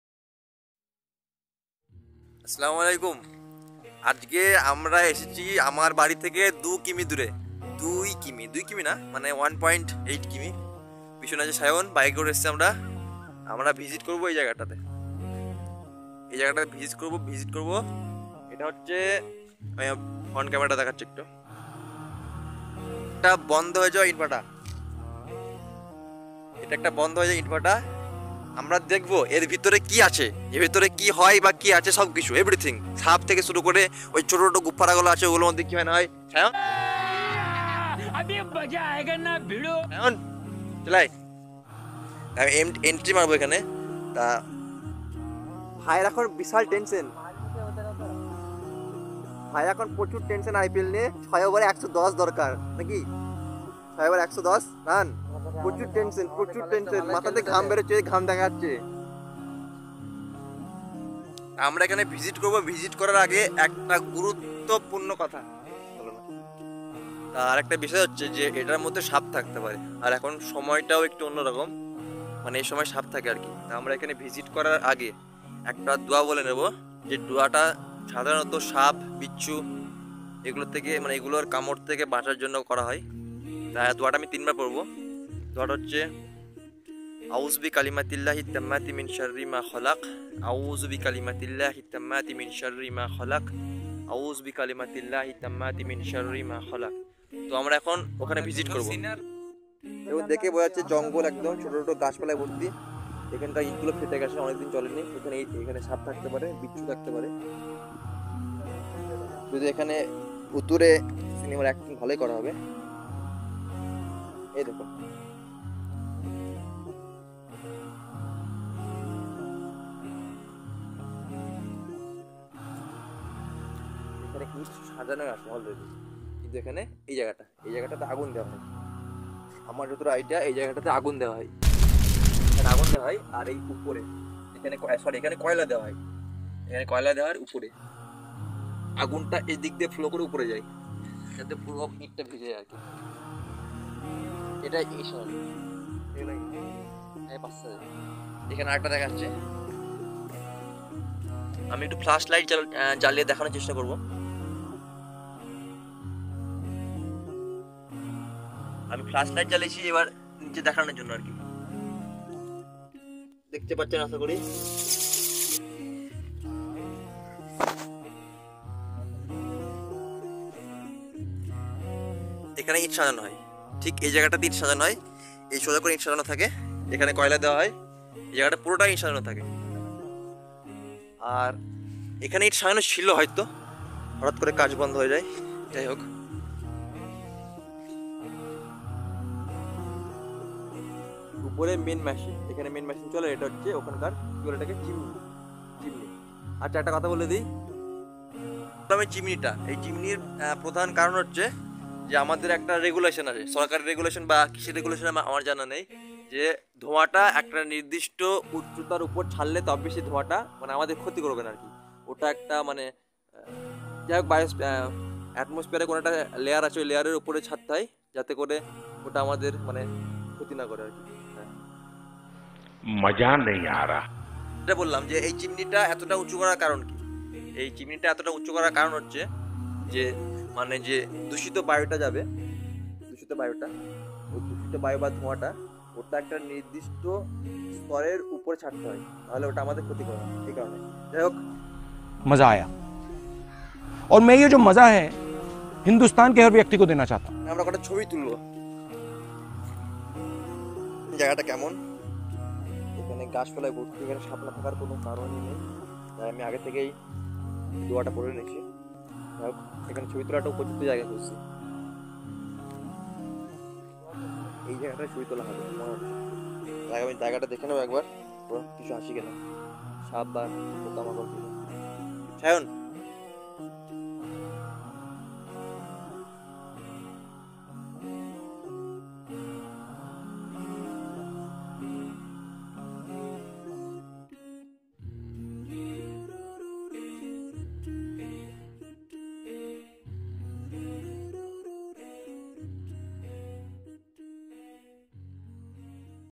Visit to Brick kiln(ইটভাটা) | Funny Mini vlog | Video
Join me on a thrilling adventure as I explore an old brick kiln! In this YouTube video, I take you on a journey through time and history as we venture into the depths of this abandoned structure. You'll see the incredible architecture and learn about the fascinating process of brick-making. With its eerie atmosphere and haunting echoes, this kiln is sure to give you chills. So, grab your flashlight and come along with me as we uncover the secrets of this forgotten relic. Don't miss out on this exciting and unforgettable experience!